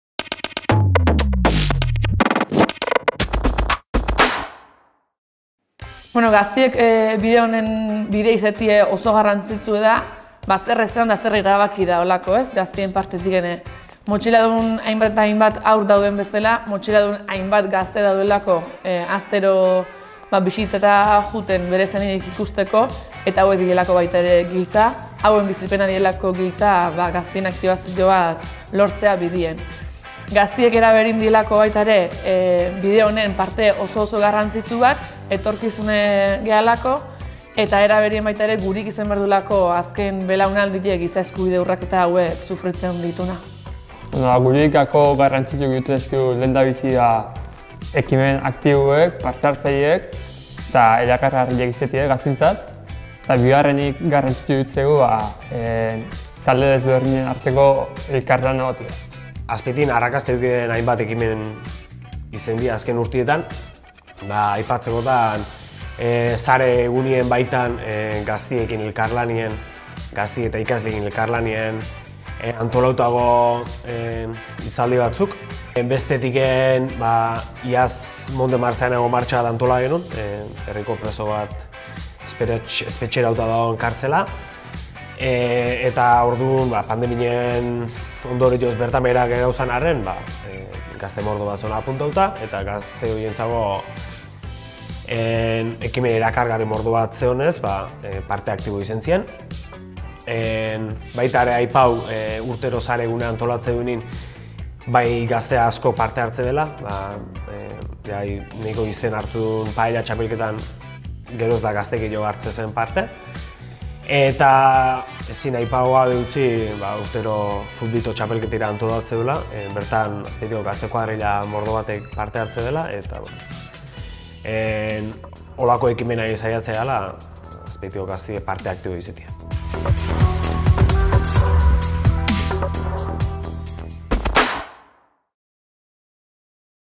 📽Herriz herri bidea gara, 5️⃣
Azpetiko Sareko kideek gazteekin egindako lanketa azaltzen dut bideo honetan. Lantxukuna egiten ari dira hauek ere. Animo!